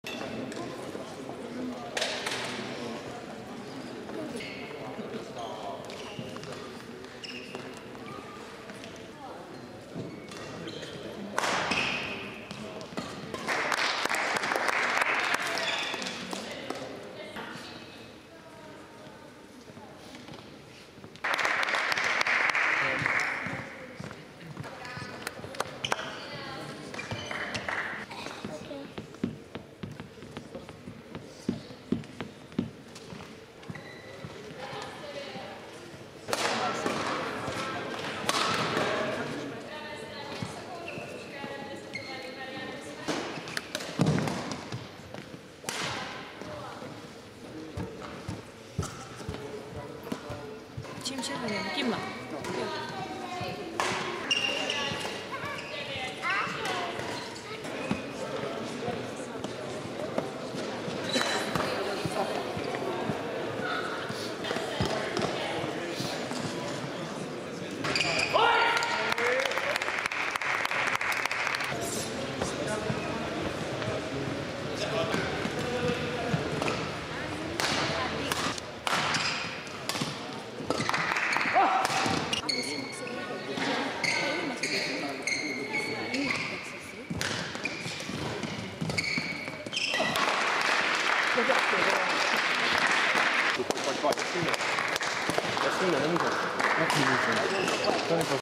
Semifinále badmintonové extraligy 2012 - Brno Bohunice
Highlights sobotního semifinálového dne.